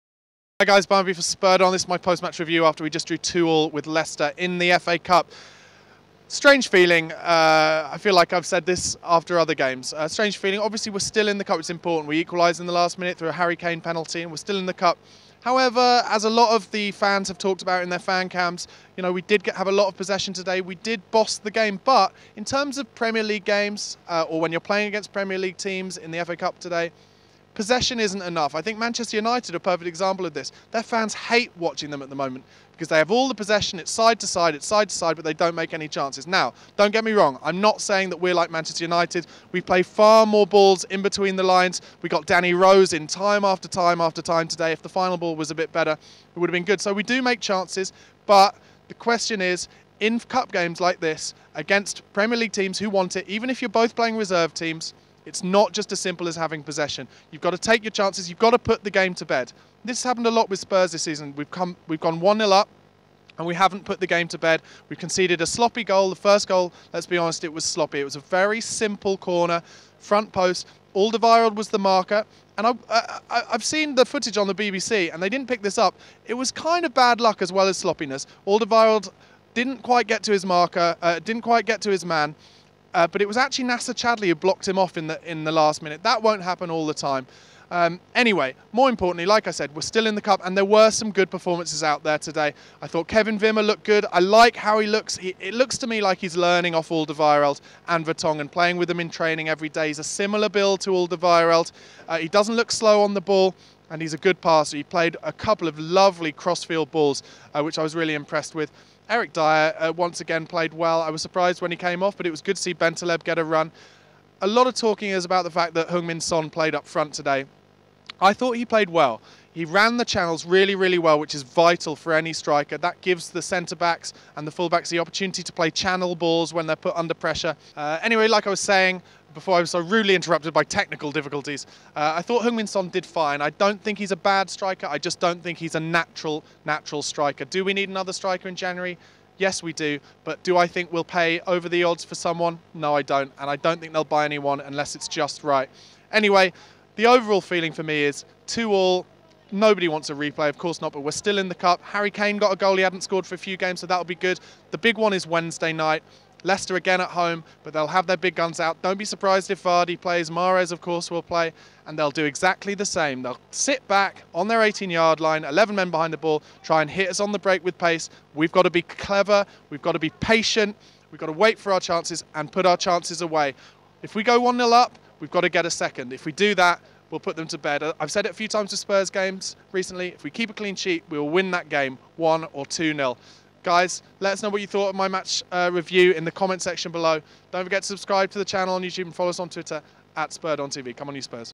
Tottenham Hotspur 2-2 Leicester | Goals: Eriksen, Kane | Match Review
Barnaby gives his Match Review after the Spurs 2-2 Leicester FA Cup game.

Facebook.com/coysnews